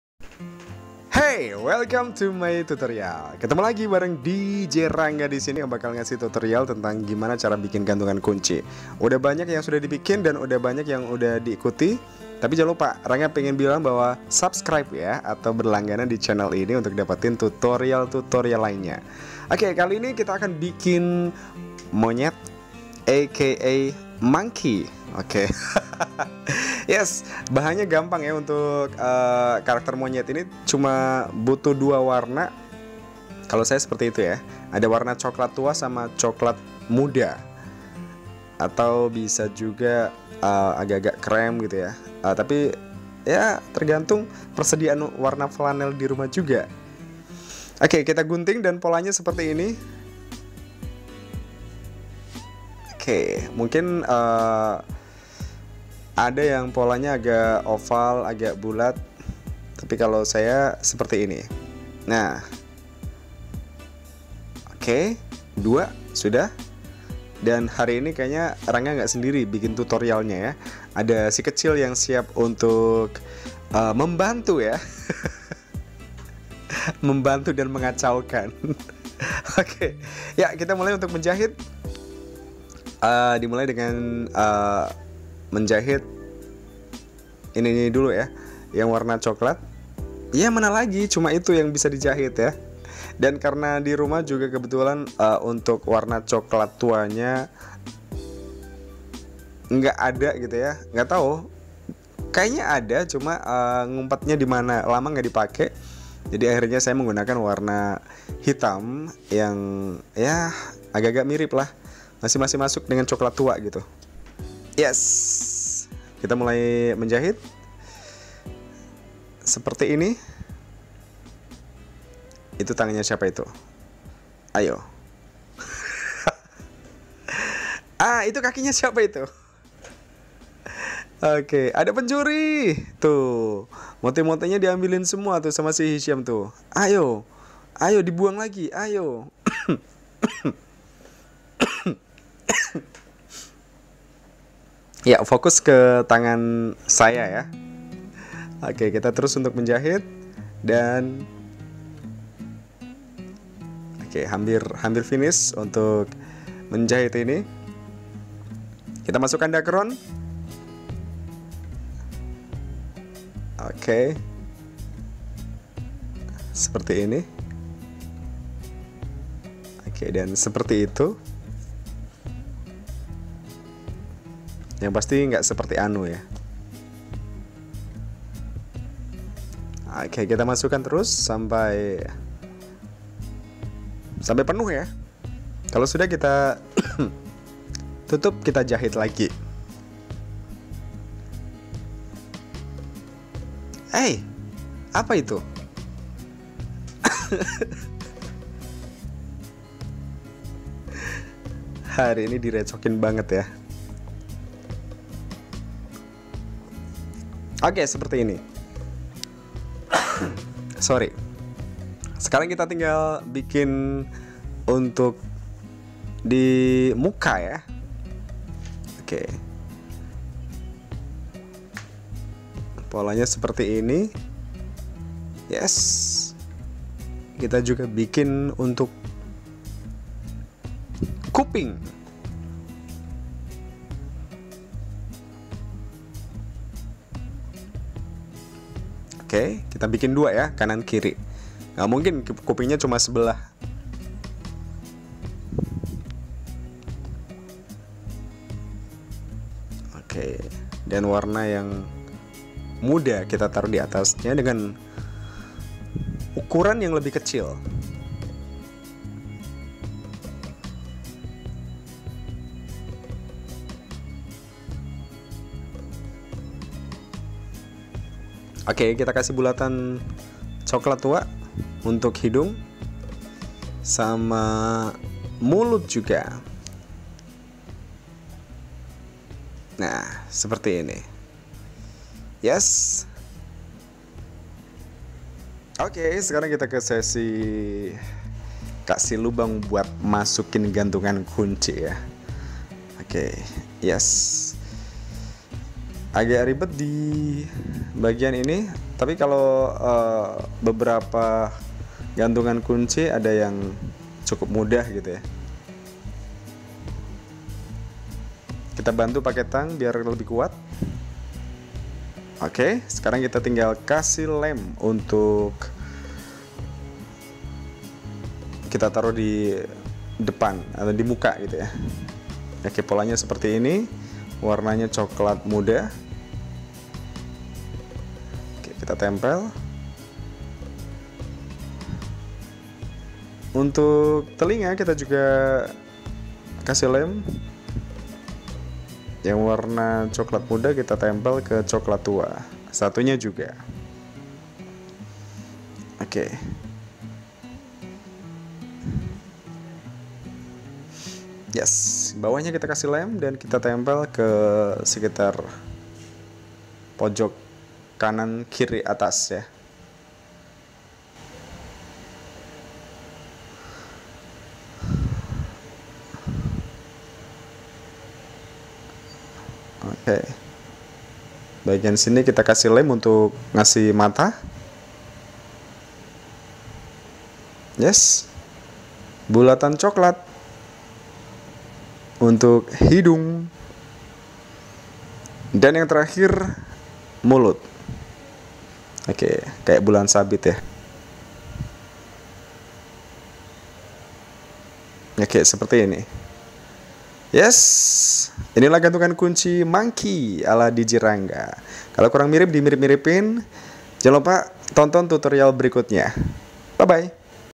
Tutorial Flanel felt | Gantungan Kunci MONYET ala rangga Bastian
Tutorial Craft Handmade dari flanel kali ini sya mencoba bikin Gantungan Kunci MONYET / MONKEY ala Rangga Bastian

Liat Juga Yukk Tutorial Gantungan CAR/ MOBIL disini ..

Info Pemesanan Gantungan Kunci/ Bross/ Pensil Flanel bisa via WA ke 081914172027